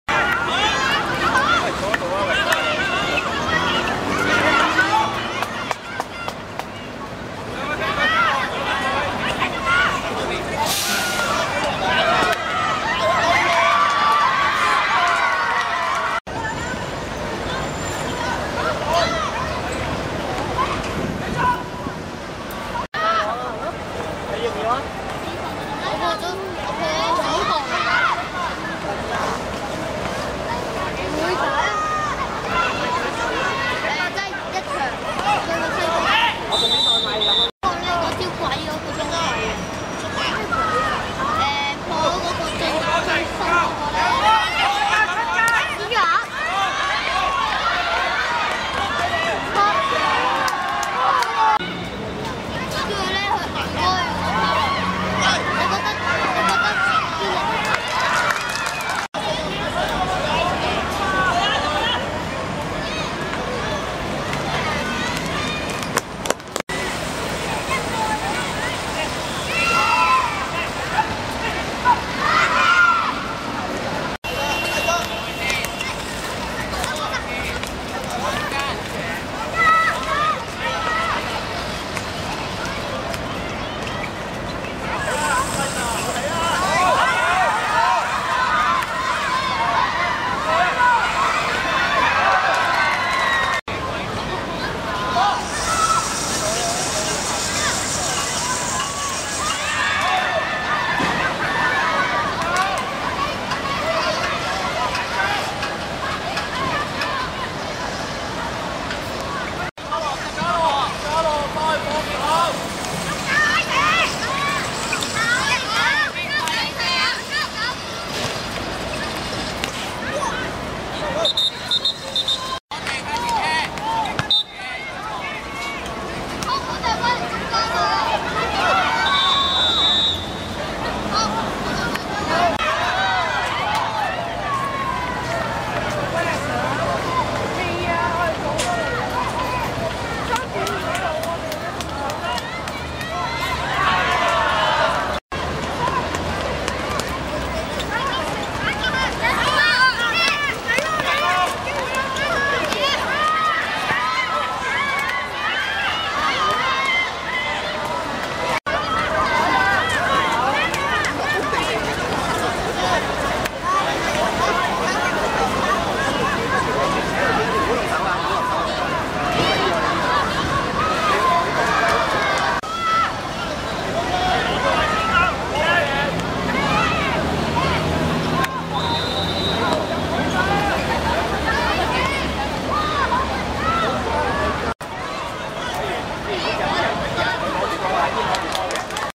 新界西(藍)vs九龍西(黃)(2012賽馬會青少年足球D組八強賽)精華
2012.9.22.葵涌足球塲 14:00 九龍西以上半塲一球入球勝出1:0晋級 其龍門表現可圈可點